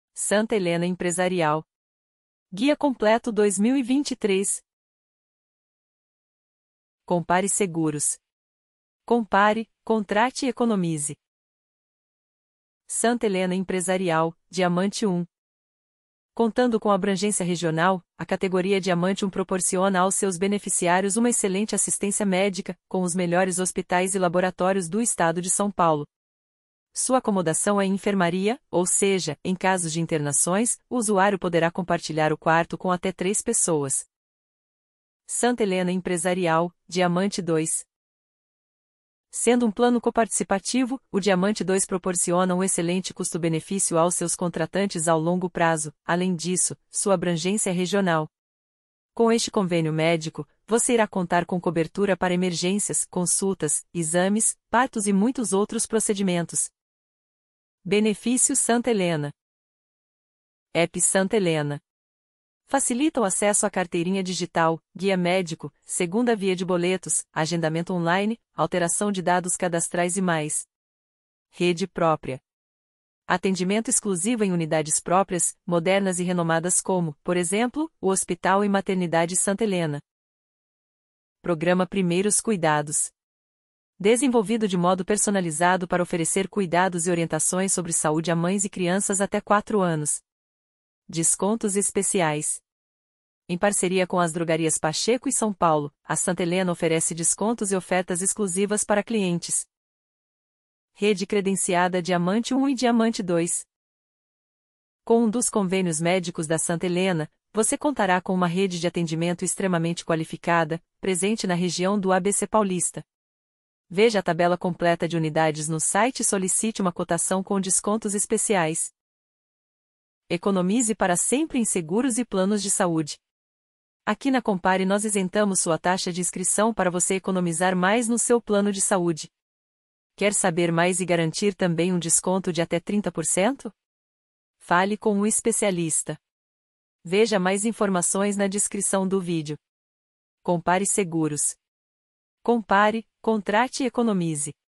Santa Helena Empresarial - Guia Completo 2023
Tempos do Vídeo:
00:00 - Introdução
00:13 - Santa Helena Empresarial / Diamante I
00:35 - Santa Helena Empresarial / Diamante II
00:58 - Benefícios Santa Helena
01:41 - Rede Credenciada